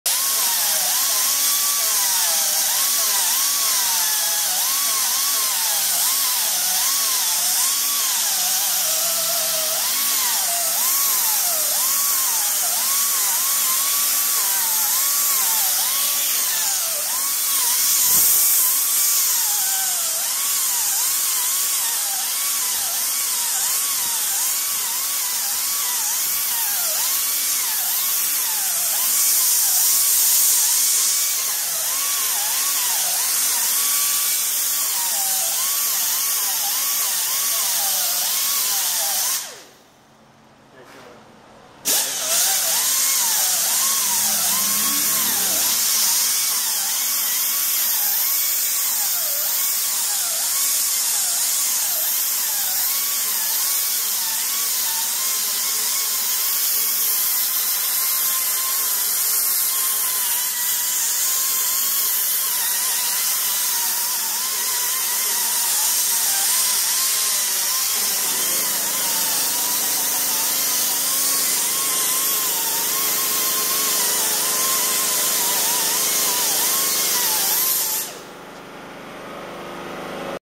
Nicholas Yust Doing Work - Episode 3 - Nicholas Forms Mountains, Hills and Valleys
Episode 3:
Watch the hands of the master doing work! Nicholas forms the epic mountains and landscape that traverse this copper masterpiece. He moves smoothly, with perfect confidence and control across a thin sheet of metal with a grinder spinning at thousands of RPMs. You can tell he's spent years perfecting his talent!